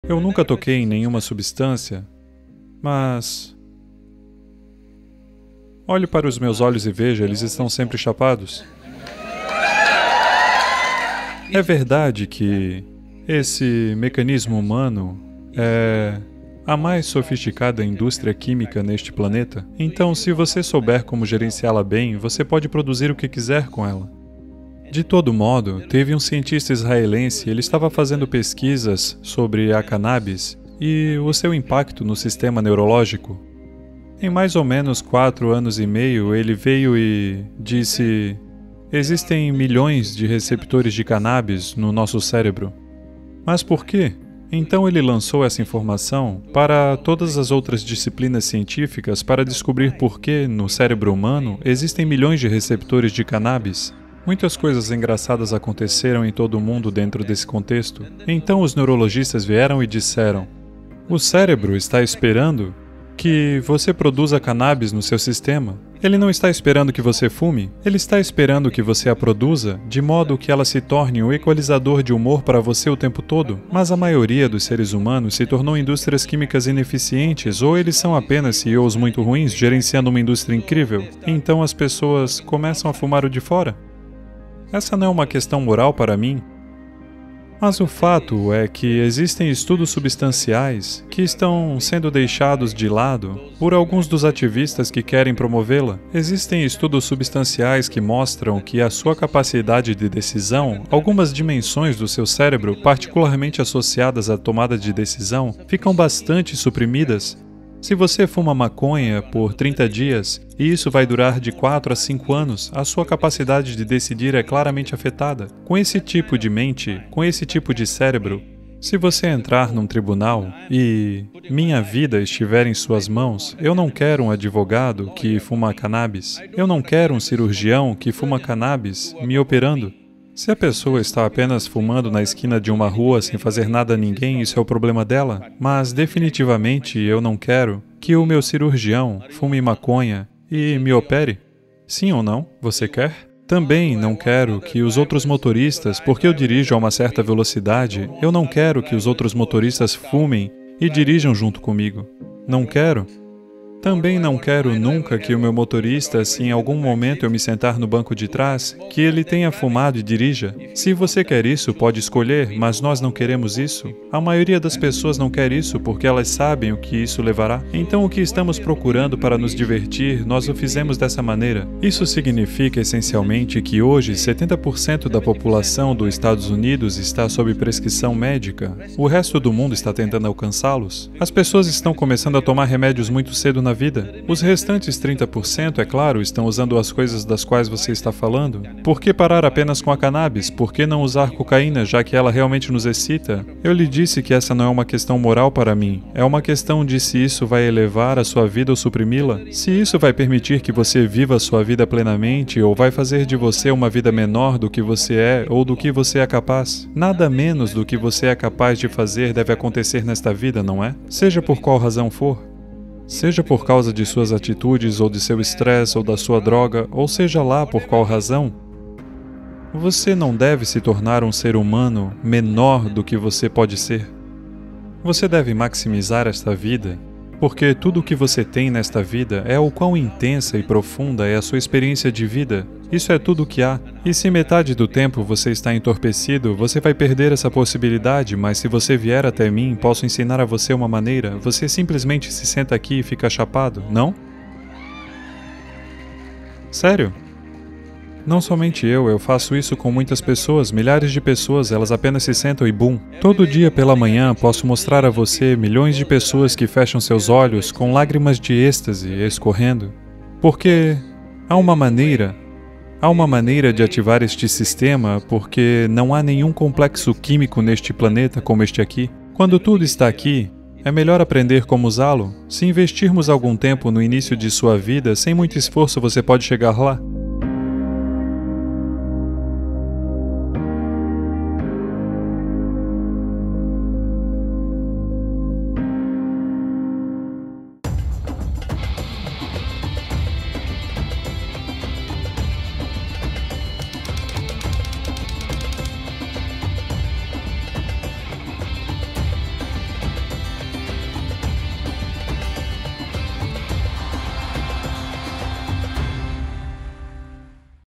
Podemos Produzir Cannabis no Cérebro? | Sadhguru Português
Neste "Unplug with Sadhguru", Sadhguru responde uma pergunta sobre se é aceitável fumar maconha e se a cannabis deveria ser legalizada.